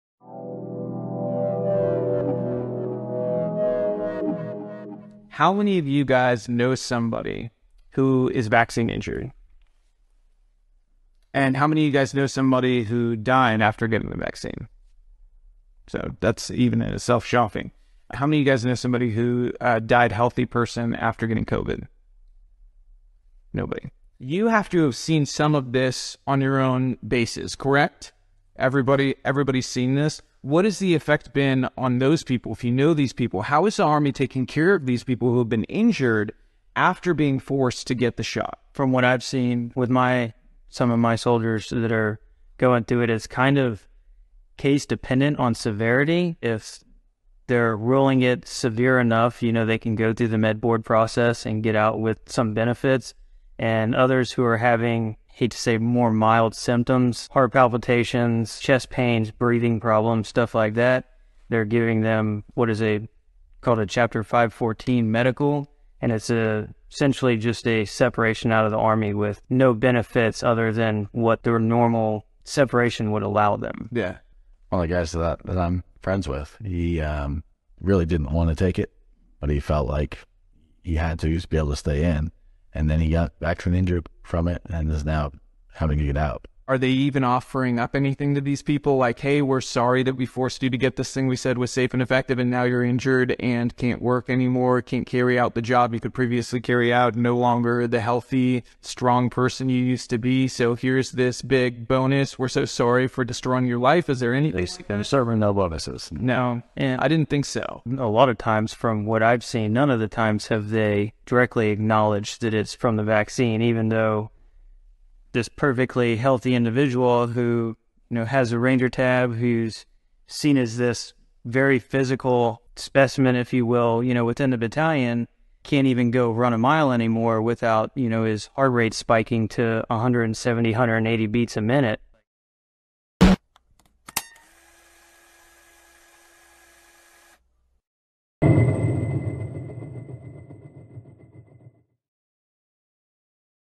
SOLDATI AMERICANI PARLANO DI PERSONE DANNEGGIATE DA VACCINO NELL'ESERCITO
I soldati americani dopo i vaccini non godono più tutti di ottima salute, è strano!

stranamente non escono mai diagnosi chiaramente correlate ai 'vaccini', pertanto si tratta della solita NoCor19.

Tuttavia sembra che in caso di invalidità  gravi i nostri eroi vengano liquidati con una buona uscita e poco altro, e possono già rendersi fortunati a non far la fine dei soldati cinadesi.

resto è un grande onore e privilegio servire il proprio paese che, da quando è NATO, fa della difesa della libertà il proprio cavallo di battaglia (libertà a condizione che sei 'vaccinato' ovviamente).